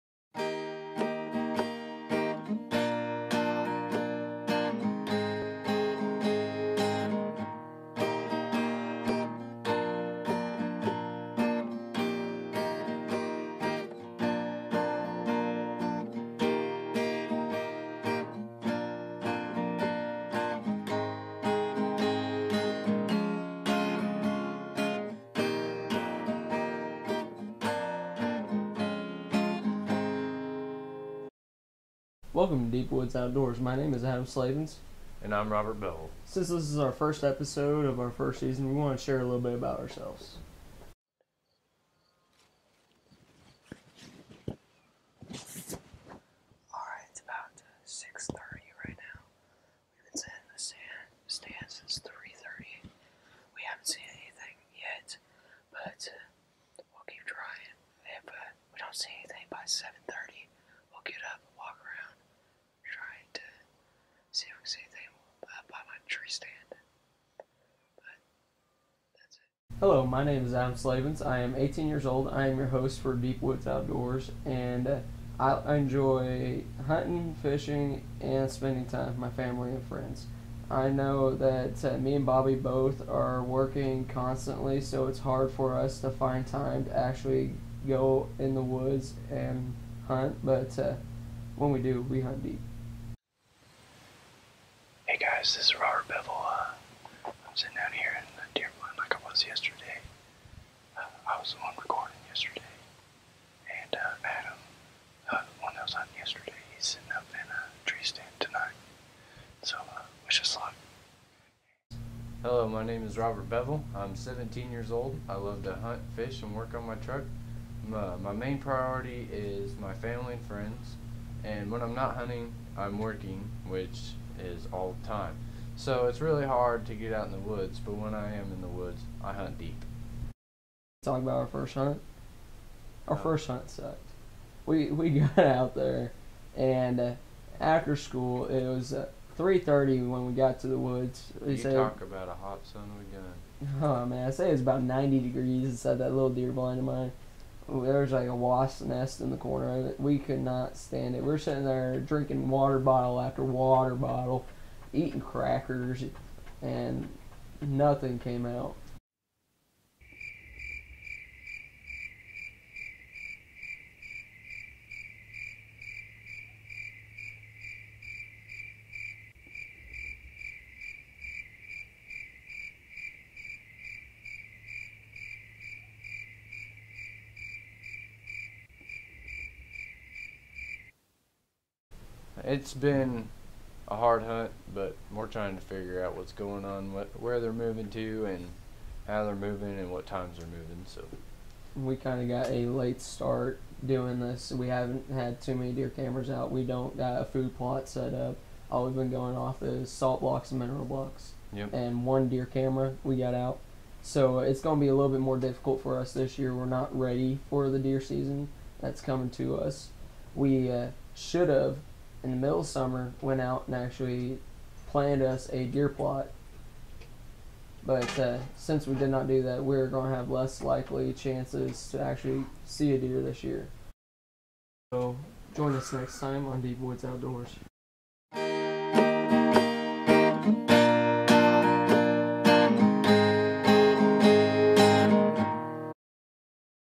Episode 1 deep woods
Our first episode is here! This video is one of a kind in our channel. This video was built with care and time. Bobby is one of the best bowmen I know. We just had a blast hunting together!